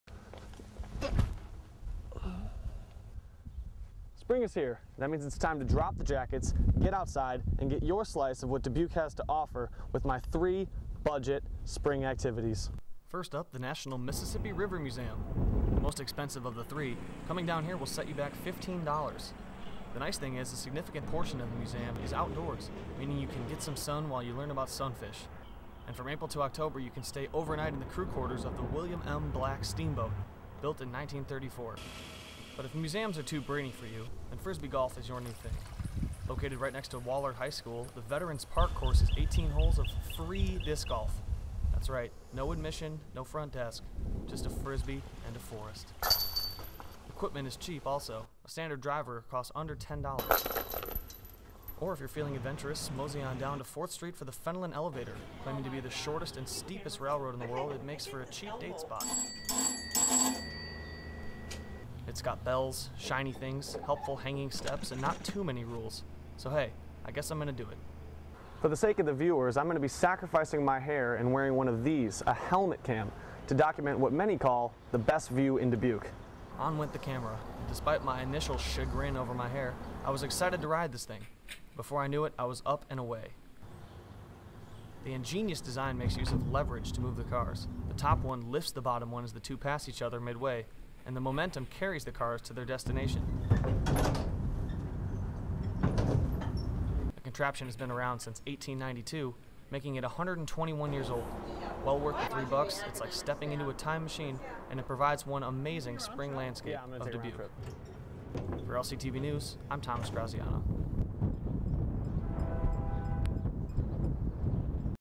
LCTV News The Stretch Spring Items
Thomas Graziano gives us a tour of Dubuque's best activities for spring weather.